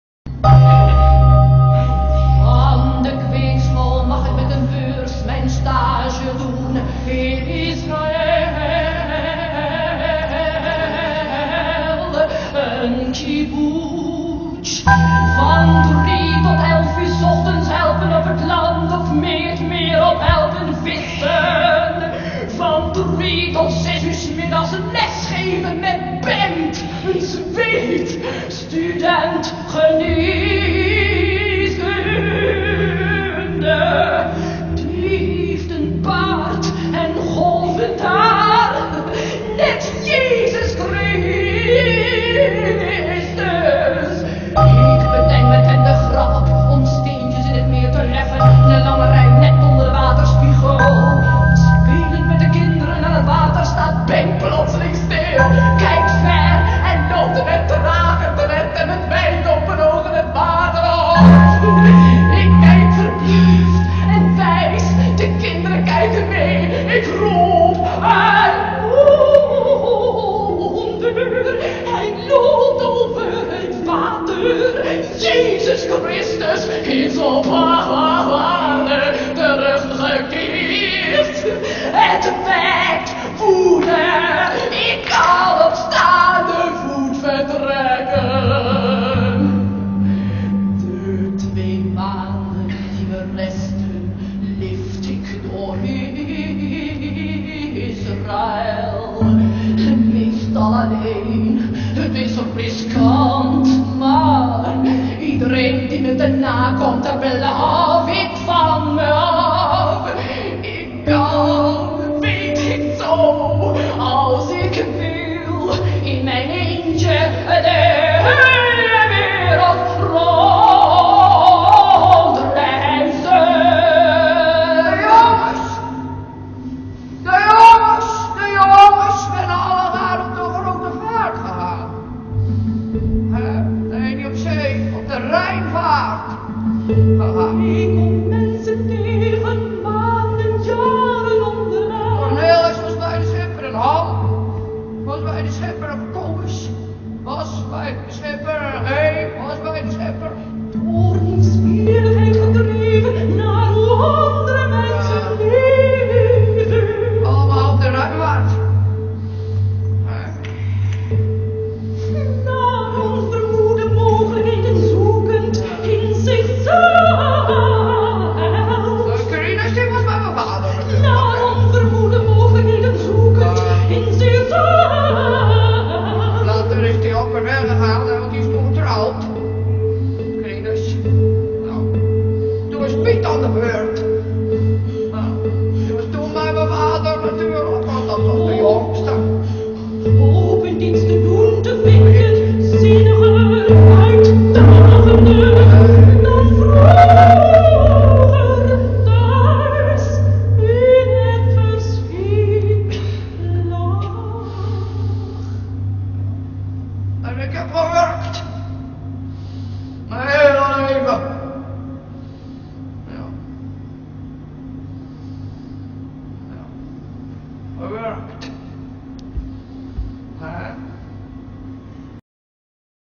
Dirty 02/09 Dieve 🔴
Content by: Theatergroep Hollandia, Paul Koek & Johan Simons (regie) / Erik Lint (video)

Description: None

Contributor Information: Cake, Paul (Director, idea and music) Simons, Johan (Director) blokdijk, Tom (text and dramaturgy) Nieuwint, Marike (author) more, Ton van der (music and synthesizer/samples), Jannie Pranger (music and Executive: Nina), Elsie de Brauw (Executive: Nel) Nadeem, Leo the (decor) IBE, Dorine (costumes), Arjen van der Grijn (grime-cut work, opinion) Lamb, Eugène (grime, cut work) camps, Ate Jan van (light) Padua, Will Jan (sound) theatergroep Hollandia (producer)